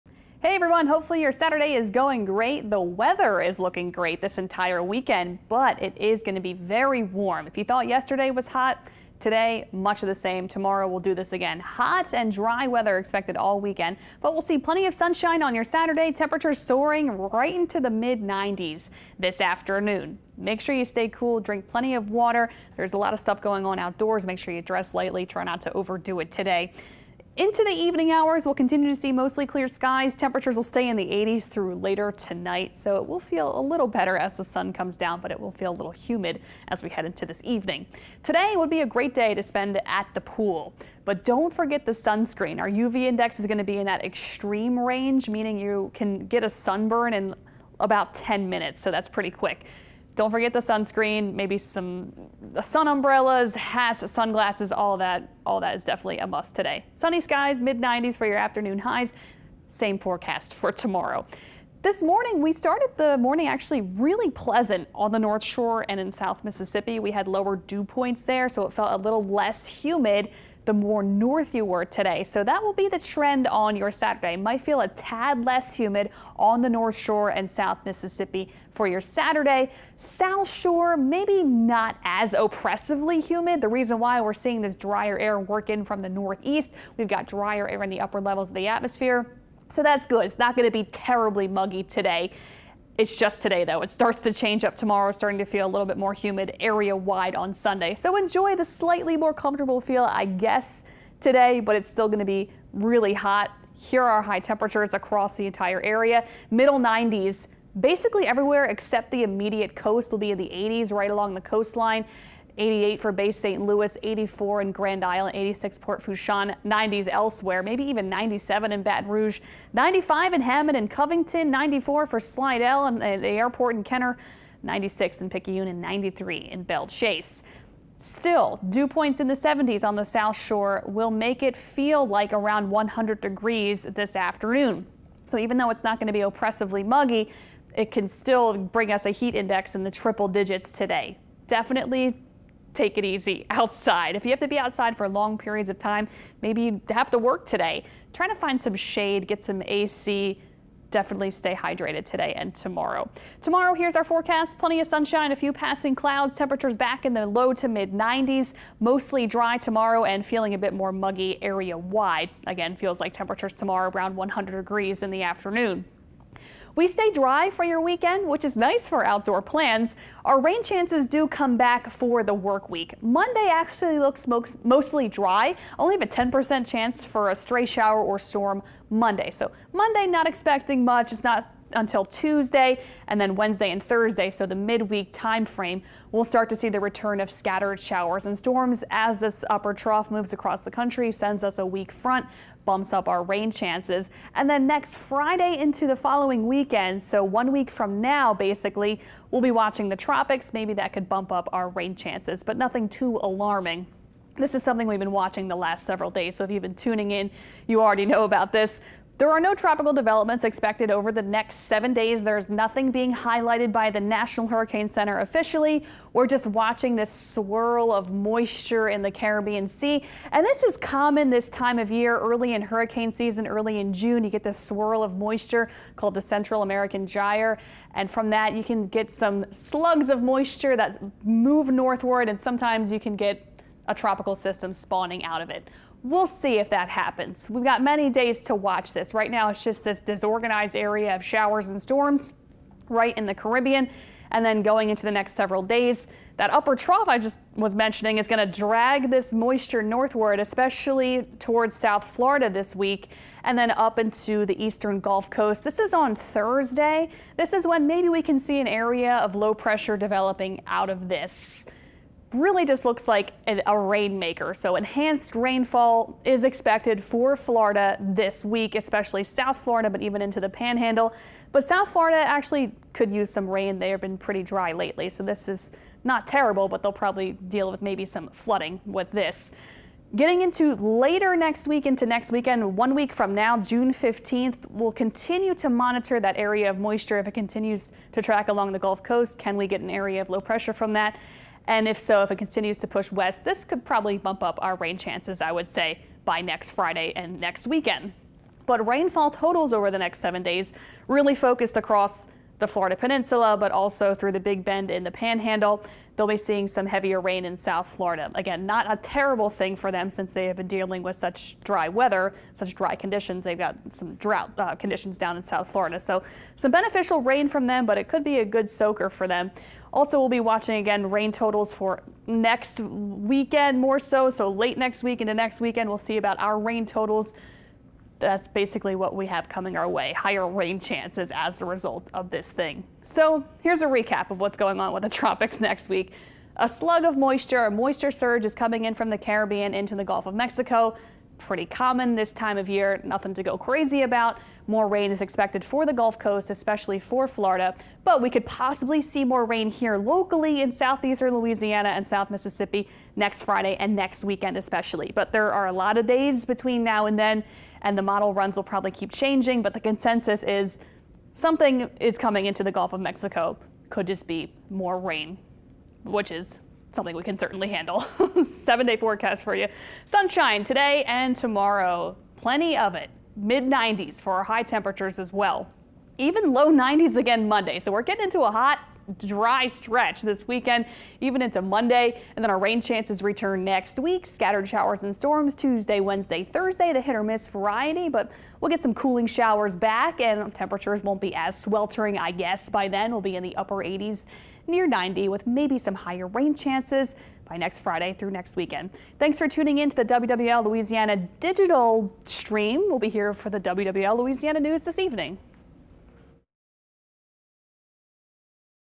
Weekend Weather Saturday, 6-8-24
Meteorologist Alexa Trischler says heat and humidity continue through the weekend.